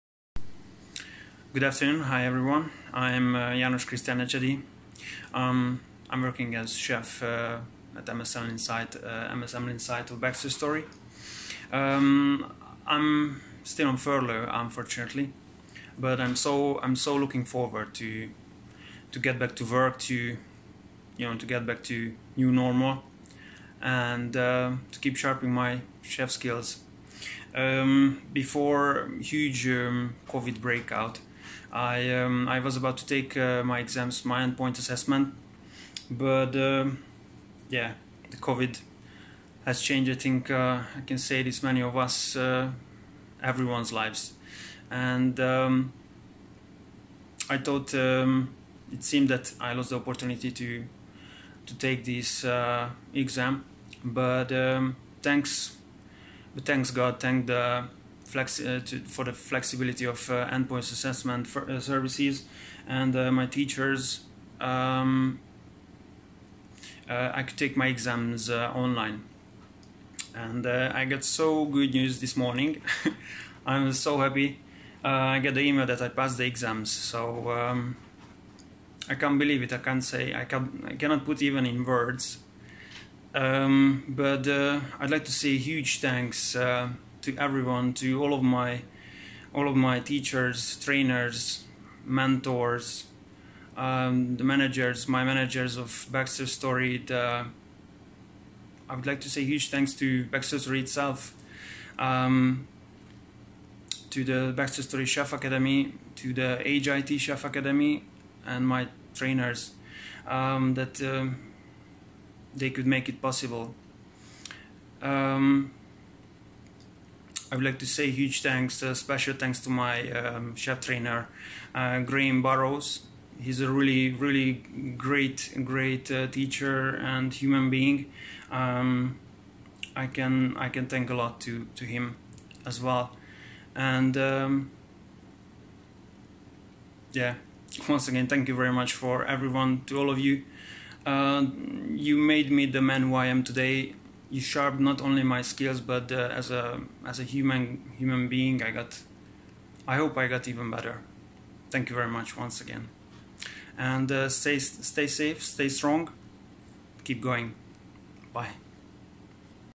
My Commis Chef Level completition 2020.07.27.
Good afternoon lovely people,

I am János Krisztián Ecsedi, chef at MS Amlin site of Baxter Storey.
I am unfortunatelly still on furlough, but I am so looking forward to get back to work as soon as possible to keep sharping my chef skills on site.
Before the huge Covid breakout I was about to take my End Point Assessment with HIT Chefacademy, but everyone’s lives has changed, seemed that means that I lost the opportunity to complete my Commis Chef Level in april.
Thanks to the flexibility of the chefacademy and the EPA services I could take my exams online and I got so good news this morning! :)
I got the email from EPA services that I passed my exam, so I have completed the Level!
I am so grateful, so happy, that I’d like to say a huge thanks to all of my mentors, teachers, chef trainers, to Baxter Storey chef academy, to HIT Chefacademy, to all my managers and superiors, that made it possible to get through successfully of the process!
Special huge thanks to my beloved HIT chef trainer, Graeme Burrows for his unconditional support and help what he gave to me. He is a really great teacher, trainer and human being!

Once again thank your very much to all of you for everything.

All the best to everyone, stay safe, stay strong!

Keep going!

Bye! ;)